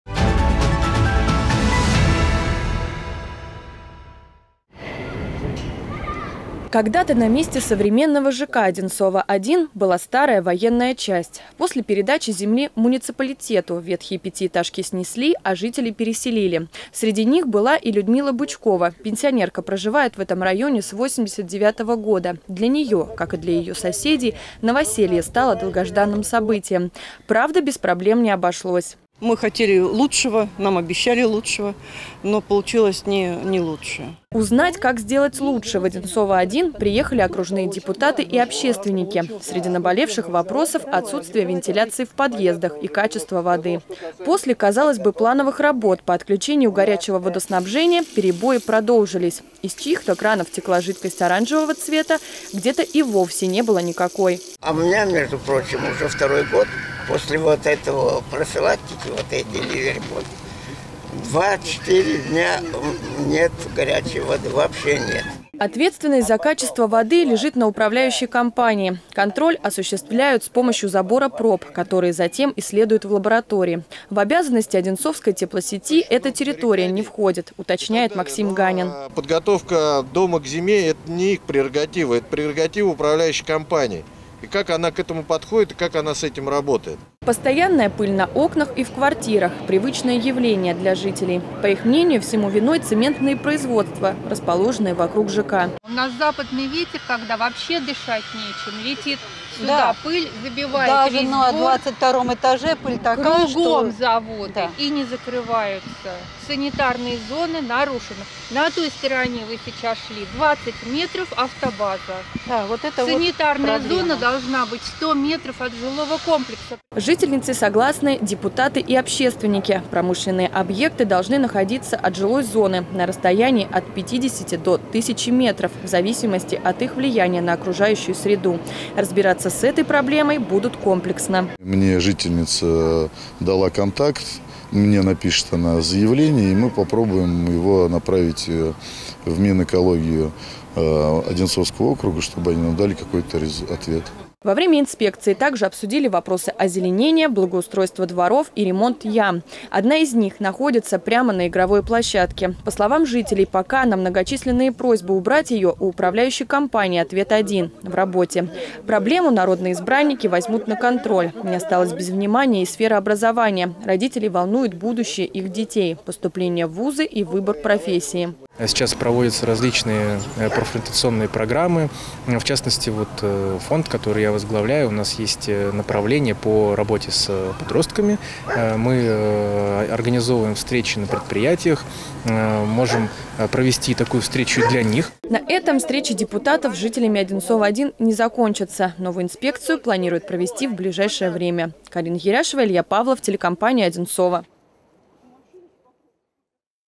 Инспекция Одинцово -1
Когда-то на месте современного ЖК «Одинцово-1» была старая военная часть. После передачи земли муниципалитету жителей переселили. Но радость от переезда омрачили бытовые проблемы. Что беспокоит вчерашних новоселов, узнала Карина Еряшева.